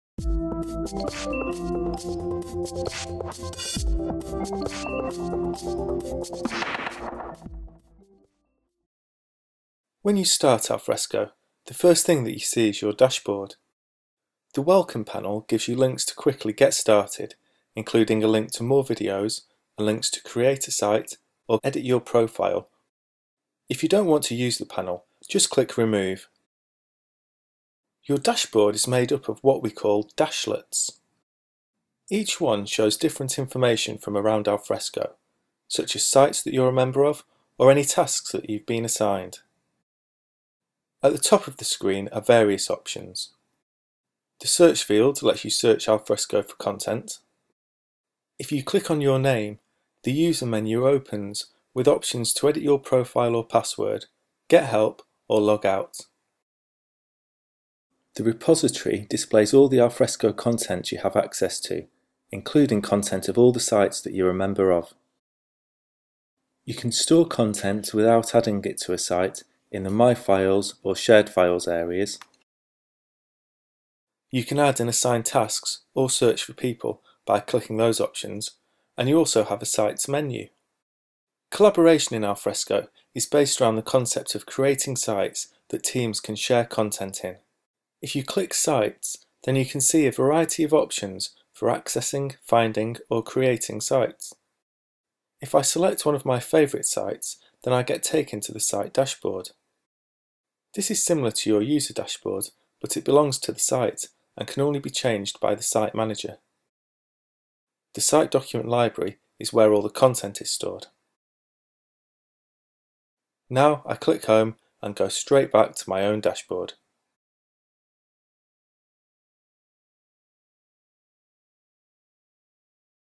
Alfresco One 4.2: Tour of Alfresco One
This Alfresco One video shows you the key features of Alfresco One.